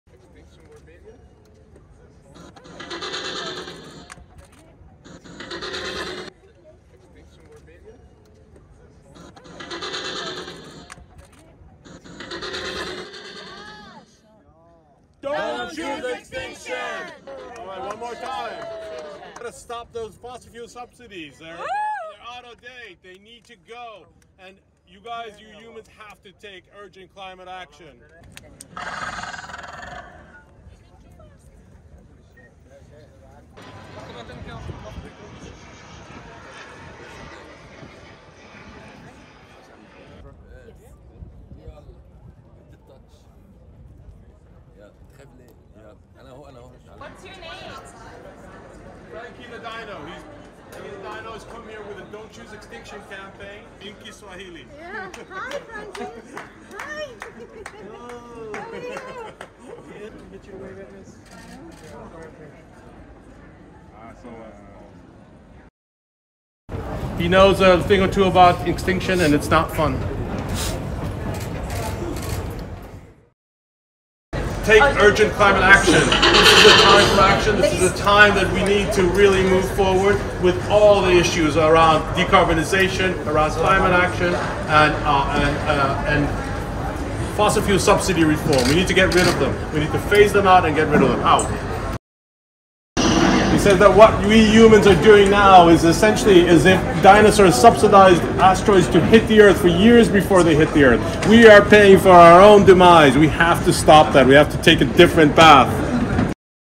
: Climate change:"Don't choose extinction”
A recent study shows that the world have already gone past 1.5°C.
An extensive analysis of SMHI's historical observations shows that Sweden's average mean temperature 1991–2020 has increased by 1.9°C compared to the period 1861–1890. This is roughly double the global average for the corresponding period (0.9 °C).

This warming and lack of climate action has certainly not gone unnoticed here in Sweden. Recently, Greta Thurnberg and 600 other young Swedes filed a class action lawsuit against the Swedish state for failing to take adequate measures to stop climate change. The lawsuit is part of an international wave of climate-related legal action, some of it targeting national governments.
Also there was Frankie the Dinosaur, the UN's climate change mascot. The Utahraptor carried a sign that read: "Don't choose extinction” to warn of the impending doom caused by the rise in emissions from burning fossil fuels, and to discouraging billions spent on fossil fuel subsidies.